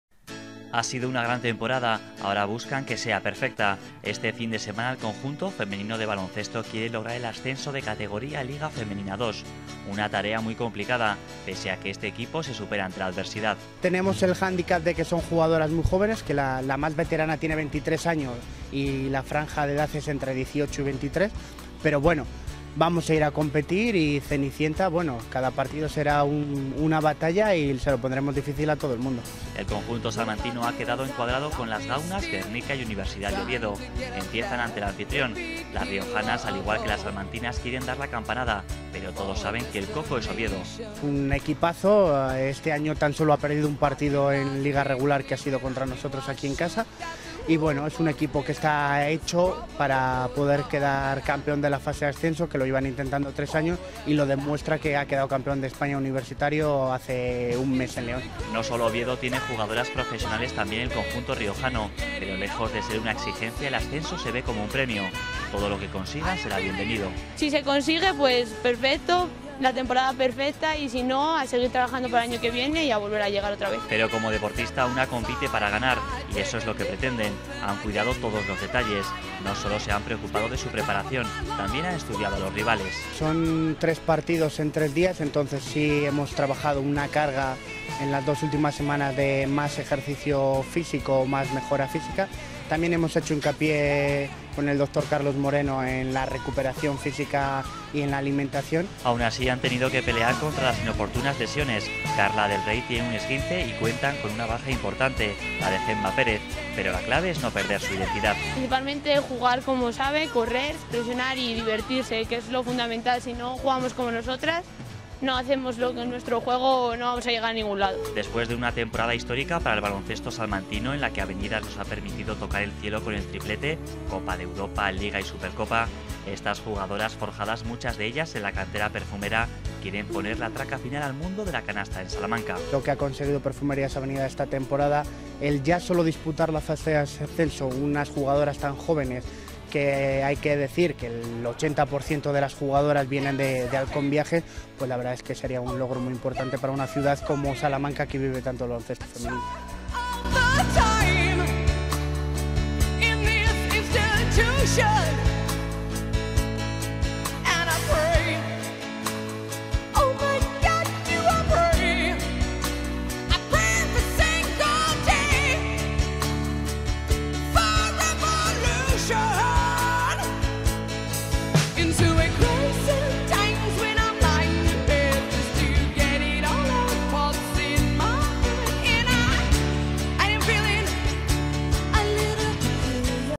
Baloncesto USAL ante la fase de ascenso a Liga Femenina 2
El equipo de baloncesto femenino de la Universidad de Salamanca disputa la fase de ascenso a Liga Femenina 2 con una enorme ilusión, a pesar de que las chicas del técnico Manolo Rodríguez no parten como favoritas.